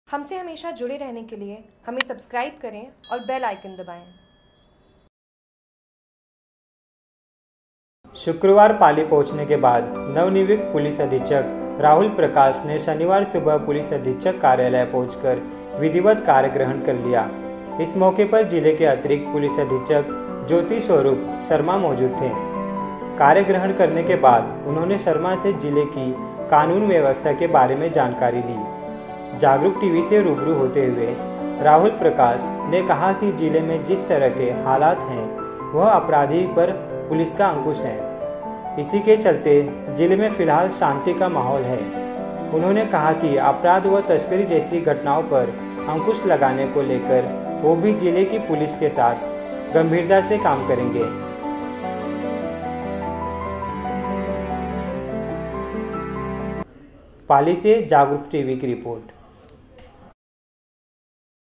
JAGRUK TV : पाली । नवनियुक्त पुलिस अधीक्षक राहुल प्रकाश ने पुलिस अधीक्षक कार्य ग्रहण कर लिया
शुक्रवार को पाली पहुंचने के बाद नवनियुक्त पुलिस अधीक्षक राहुल प्रकाश ने शनिवार सुबह पुलिस अधीक्षक कार्यालय पहुंच कर विधिवत कार्य ग्रहण कर लिया इस मौके पर जिले के अतिरिक्त पुलिस अधीक्षक ज्योति स्वरूप शर्मा मौजूद थे ।